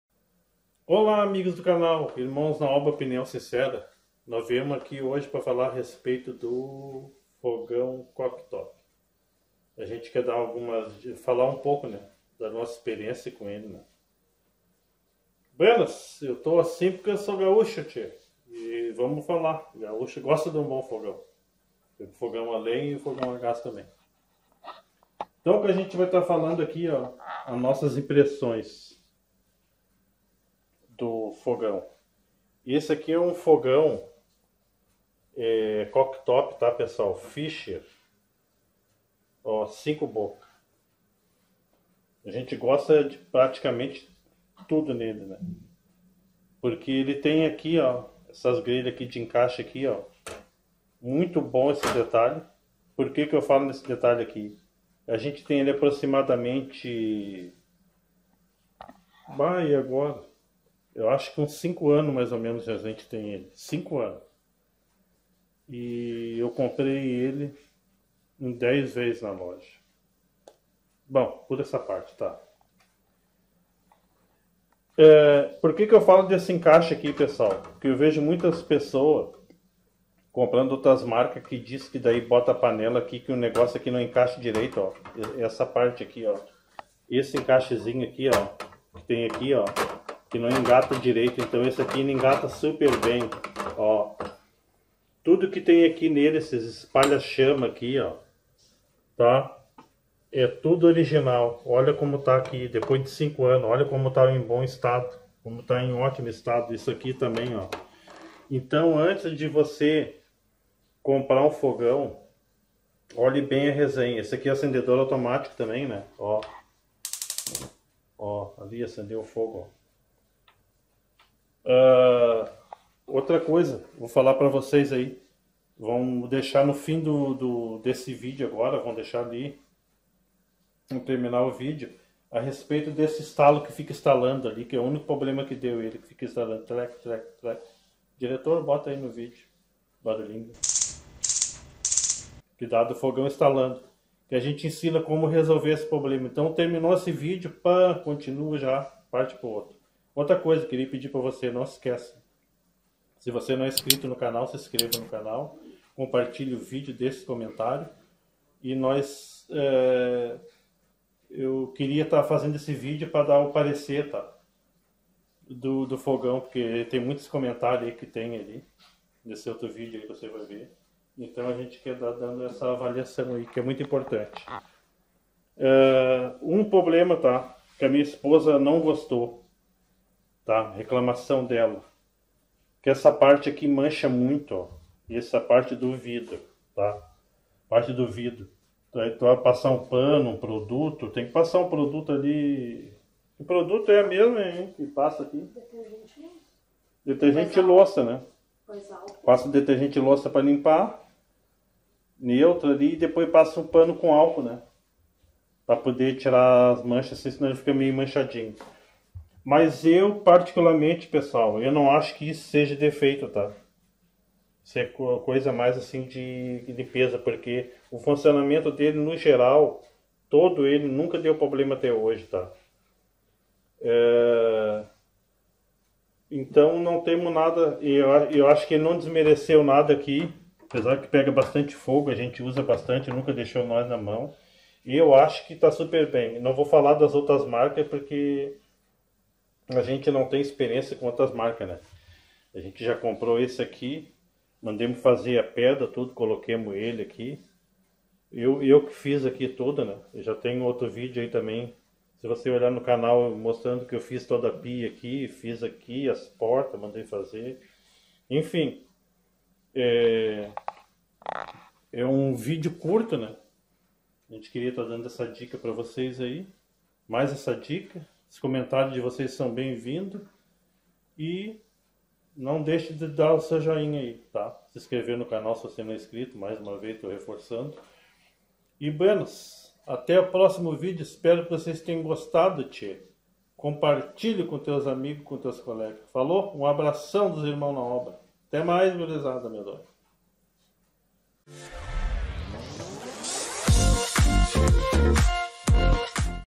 Resenha Fogão Cooktop Fischer
@irmãosnaobraopinoãosincera Neste Vídeo estamos mostrando a resenha do fogão cooktop Fischer de 5 bocas, nós temos este fogão a mais de 5 anos, gostamos muito dele, não deu problema, (apenas estalos do acendedor) conforme o vídeo Resolvendo Estalos no Fischer.